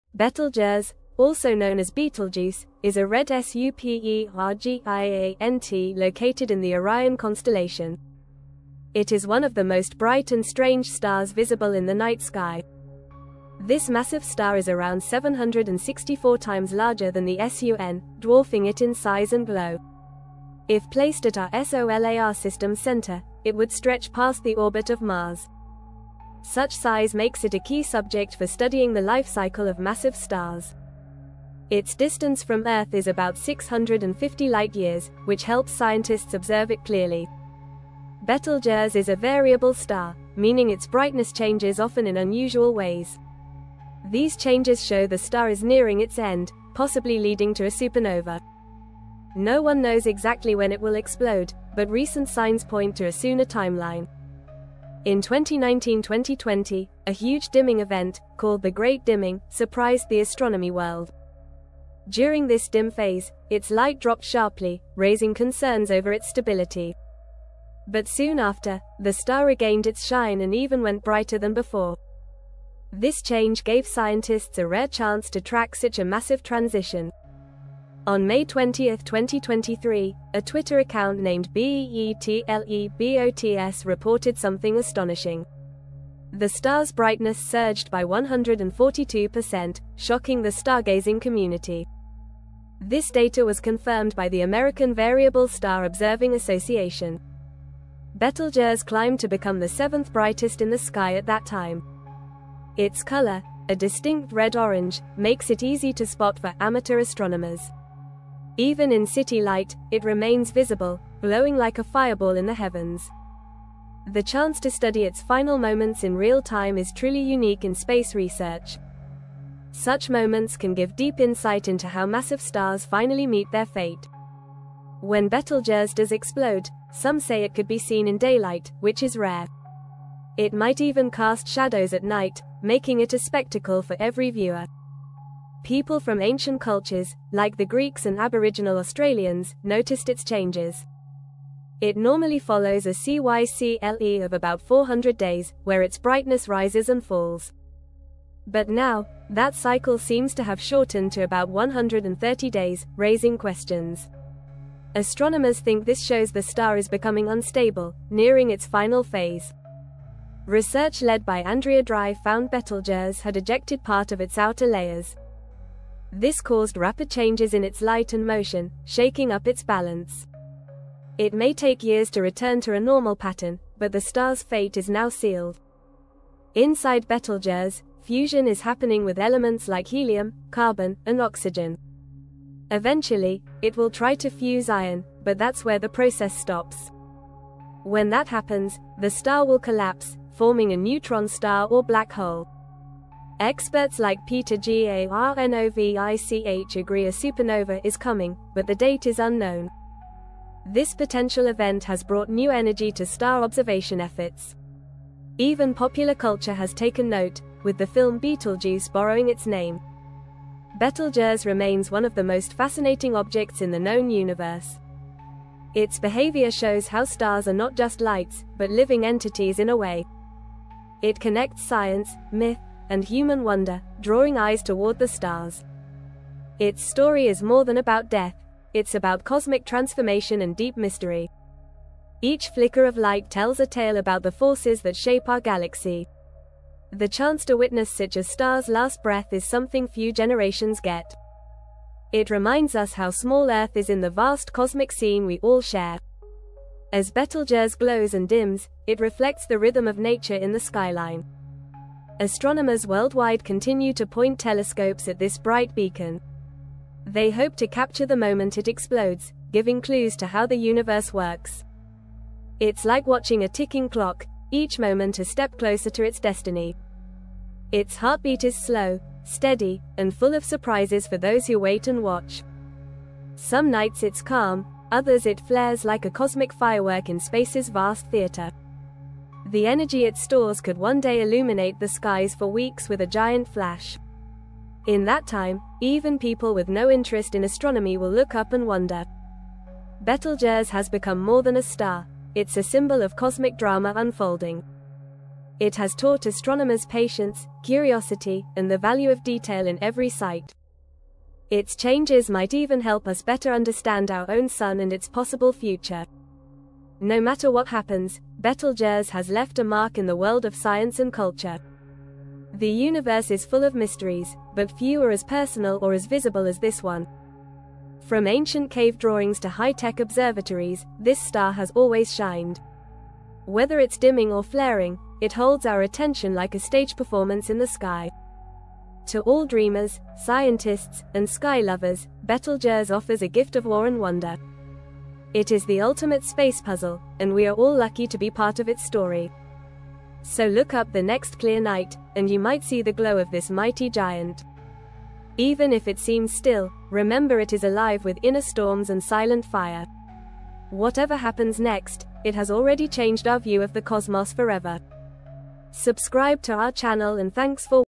James Webb Telescope Just Detected A Sudden 250 Increase In Betelgeuses Brightness!
Witness the amazing story of Betelgeuse 🌟, the mysterious red supergiant star in Orion’s constellation. It’s huge, unpredictable, and might go supernova any time! From The Great Dimming to becoming one of the brightest stars in the sky, this space giant has astronomers and stargazers super excited 🔭✨.

In this video, we explain how Betelgeuse’s strange behavior might mean it's nearing the end of its life. Learn about its brightness changes, wild eruptions, and how it could shine so bright that it’s visible during the day! 🌌 Whether you love science, stars, or just cool space stuff, this video is made for you.